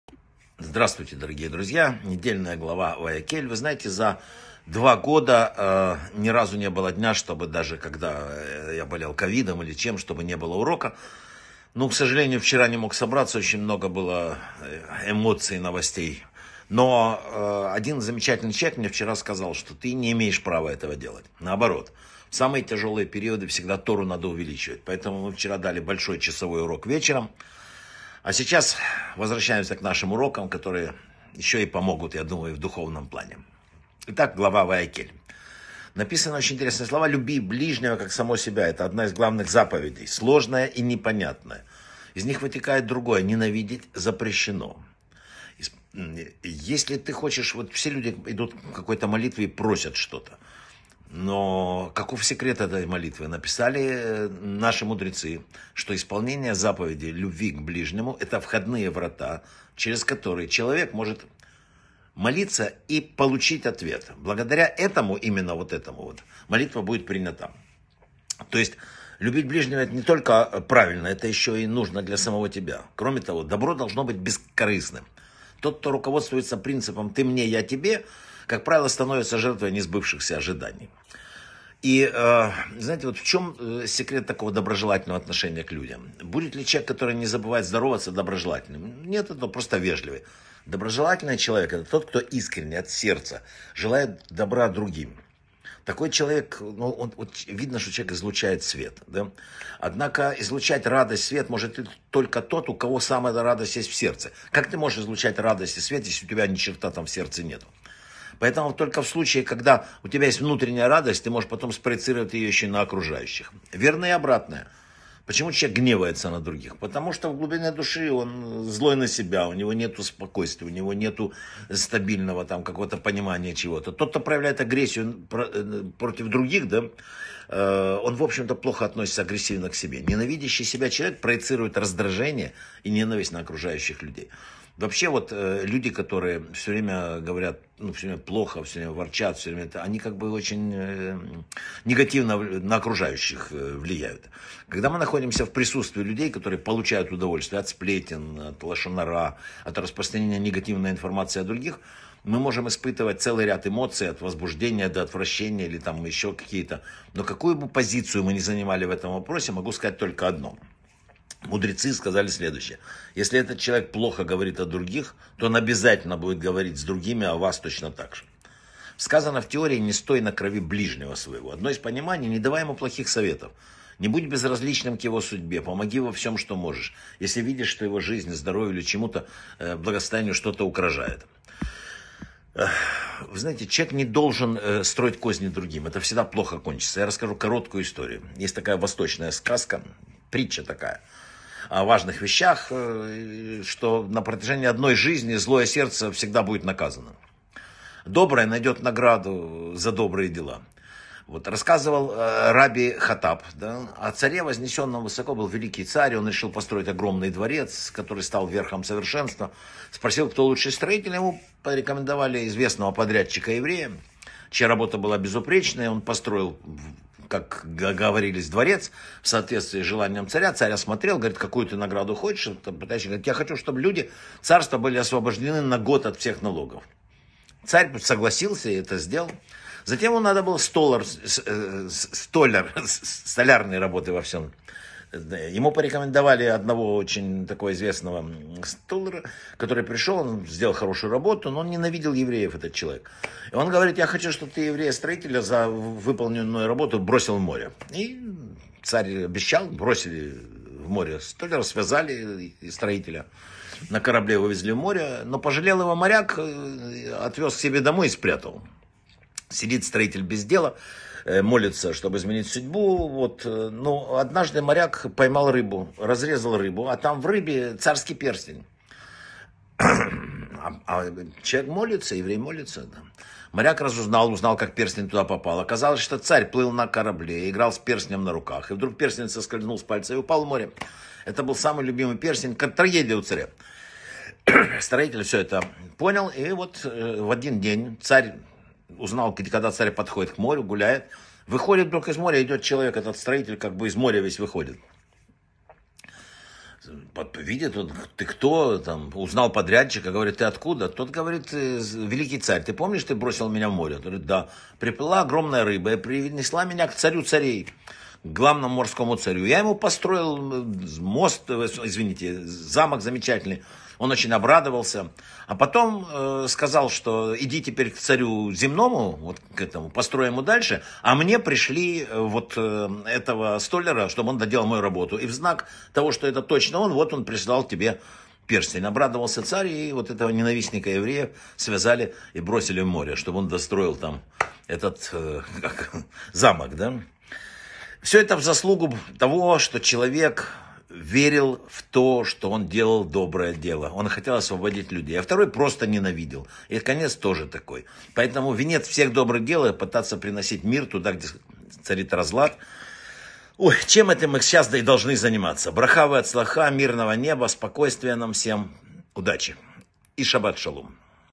В. Рабинович: Человек должен любить ближнего и нести только добро. Недельная глава Ваякгель
Заполните короткую анкету (3 минуты) и сразу откройте для себя:
📖 Начните путешествие к мудрости уже сегодня! Сказано человеку: "Люби ближнего как самого себя". Что проистекает из этой важнейшей заповеди?

В чем секрет исполнения молитвы?

Добро должно быть бескорыстным.

Какой человек способен проявлять радость и что внутри у того, кто гневается на окружающих?

"Не стой на крови ближнего своего" - один из аспектов понимания этих слов.

История о царе, строителе и столяре. Как добро возвращается человеку в заслугу его добрых дел, а зло получает наказание.